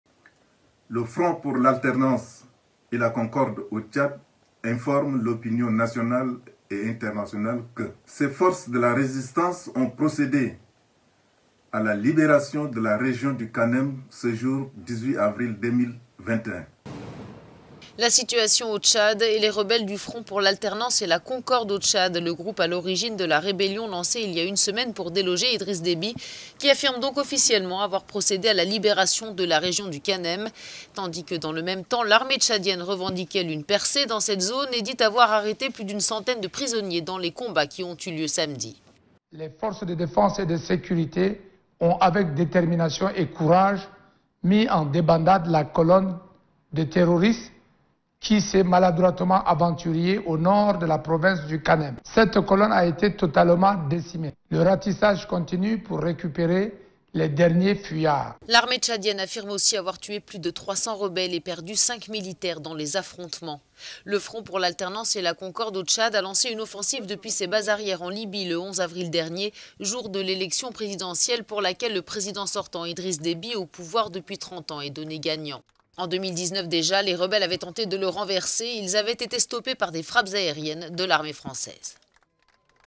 Tchad : plus de 300 rebelles et cinq militaires tués dans le Nord
L'armée tchadienne a affirmé lundi avoir tué plus de 300 rebelles qui mènent une incursion depuis huit jours dans le nord du pays et perdu cinq militaires dans des combats samedi.